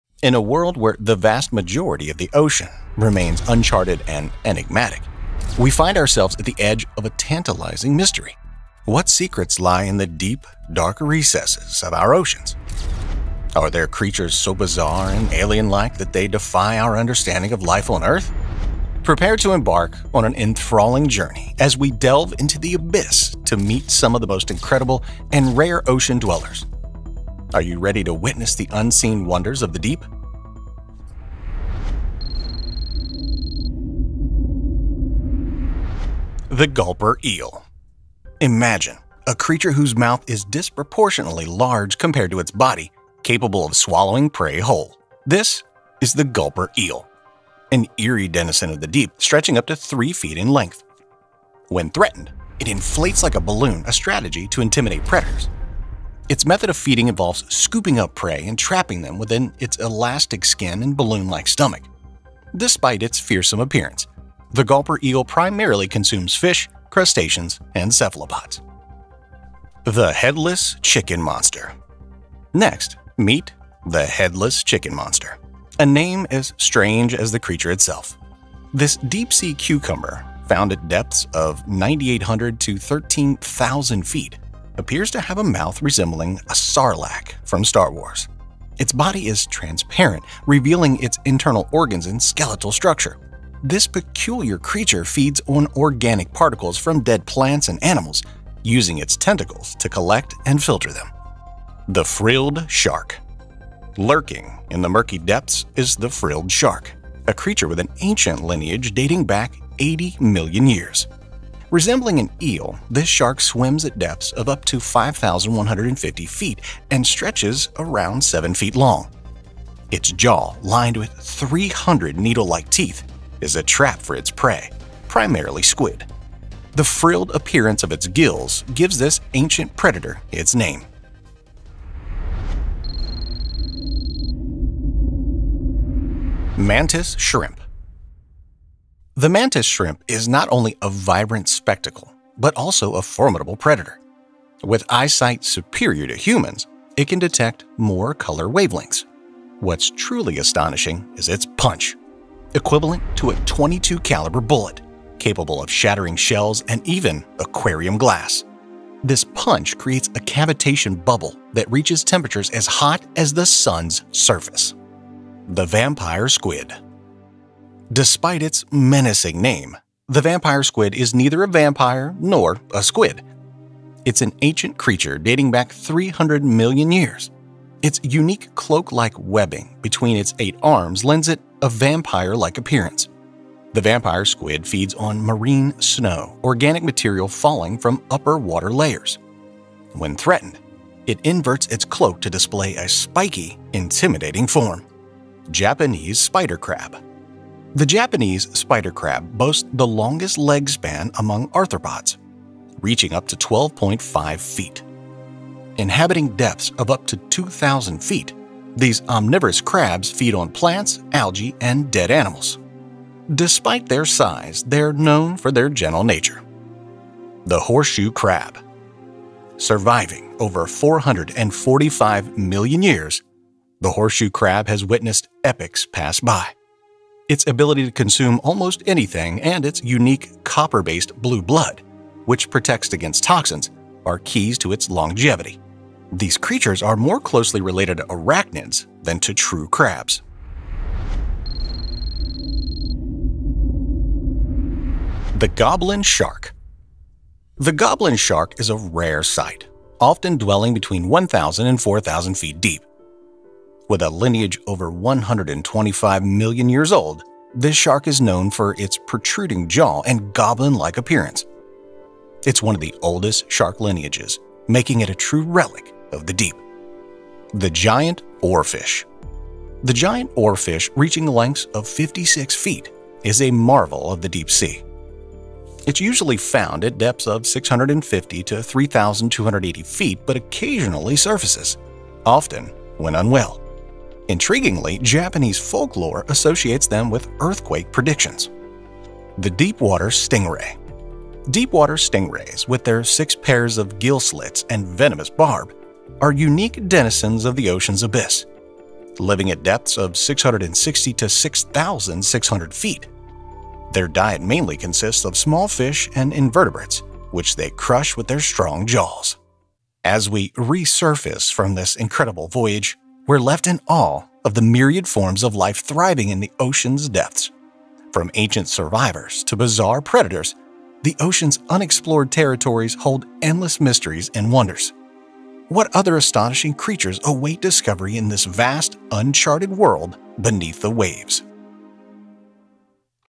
The Most Alien-Like Creatures of the Deep Sea You've Never Seen Before
In a world where the vast majority of the ocean remains uncharted and enigmatic, we find ourselves at the edge of a tantalizing mystery. What secrets lie in the deep, dark recesses of our oceans? Are there creatures so bizarre and alien-like that they defy our understanding of life on Earth? Prepare to embark on an enthralling journey as we delve into the abyss to meet some of the most incredible and rare ocean dwellers. Are you ready to witness the unseen wonders of the deep?

Dive into the uncharted depths of the ocean with our latest video, "The Most Alien-Like Creatures of the Deep Sea You've Never Seen Before." Explore the mysterious and rarely seen inhabitants of the deep sea, from the bizarre Gulper Eel to the ancient Horseshoe Crab. Our journey takes us through three captivating chapters, each revealing incredible creatures that defy our understanding of life on Earth. Witness these alien-like beings in their natural habitat and learn about their unique adaptations for survival in the deep. Whether you're a marine biology enthusiast or simply fascinated by the wonders of the natural world, this video is a must-watch!

Join us as we uncover the secrets of the ocean's depths, showcasing stunning footage and intriguing facts about these remarkable creatures. From the eerie Frilled Shark to the elusive Giant Oarfish, each discovery offers a glimpse into a world far removed from our own. Don't miss this opportunity to witness the unseen and expand your knowledge of our planet's most mysterious realm.

📌 Time Stamps:
0:00 Intro
0:31 The Gulper Eel
1:09 The Headless Chicken Monster
1:41 The Frilled Shark
2:17 Mantis Shrimp
2:45 The Vampire Squid
3:15 Japanese Spider Crab
3:37 The Horseshoe Crab
4:05 The Goblin Shark
4:27 The Giant Oarfish
4:49 The Deepwater Stingray

✅ The Alien-like Inhabitants of the Deep: Discover creatures like the Gulper Eel and the Headless Chicken Monster, showcasing their unique features and survival strategies.
✅ The Unusual and the Ancient: Learn about the Mantis Shrimp's incredible punch and the ancient lineage of the Vampire Squid.
✅ Rare Ocean Dwellers: Unveil the secrets of the Goblin Shark and the Giant Oarfish, some of the most elusive creatures of the deep sea.
✅ Adaptations for Deep Sea Survival: Understand how these creatures have evolved to thrive in extreme ocean depths.
✅ Conservation and the Future: Reflect on the importance of preserving these mysterious ecosystems and what future explorations might reveal.